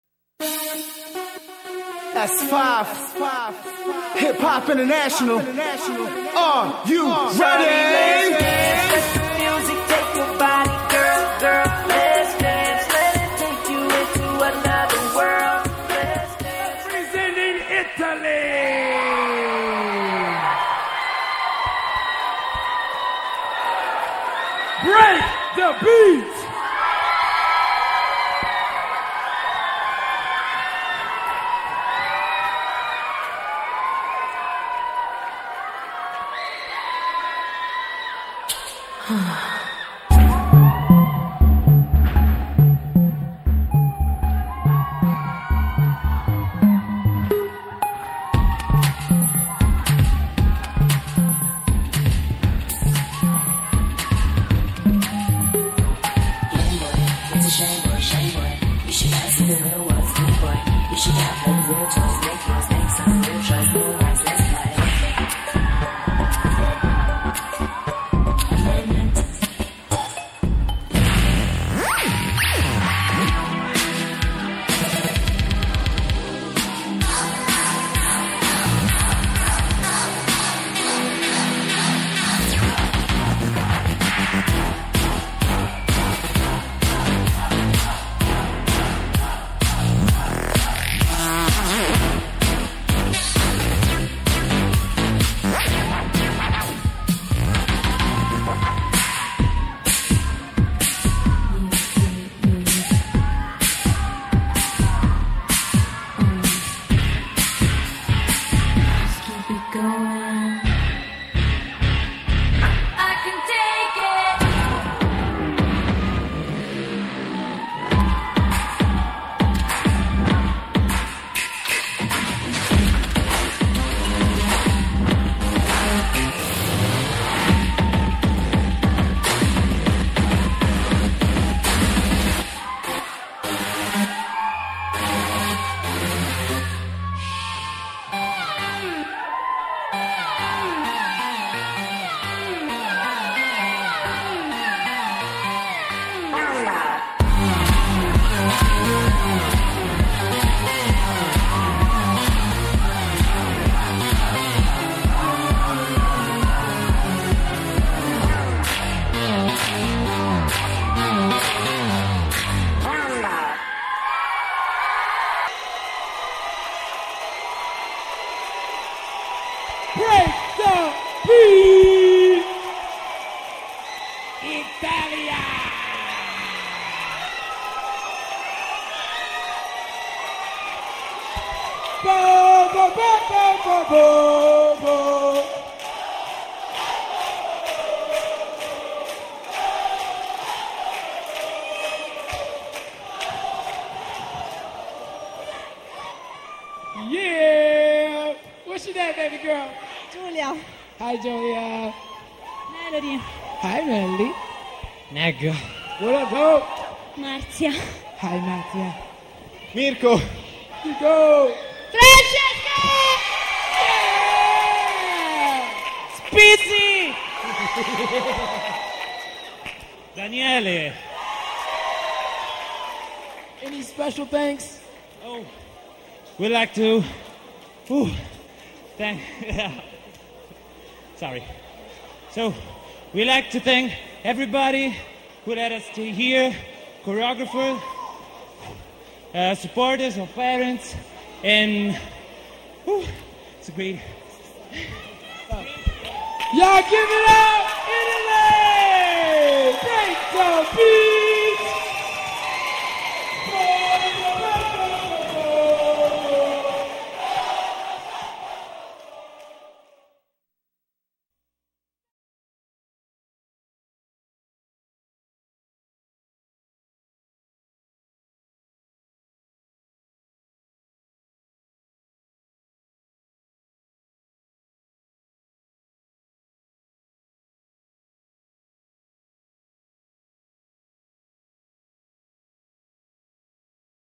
Break da Beat - World Hip Hop Championship 2010
Break da Beat (Italy) @ World Hip Hop Championship 2010
Semi-Finals  Adult Division
Friday, July 30, 2010
LAS VEGAS , USA

Choreographer : Emanuele Battista aka BIG
Dancers: Giulia salvatore- Marzia Rognini-Daniele Capogrosso- Mirko Cibba- Negassi Melke- Francesca Guerrini- Melory Tolisano- Dario Sperduto aka Spizzy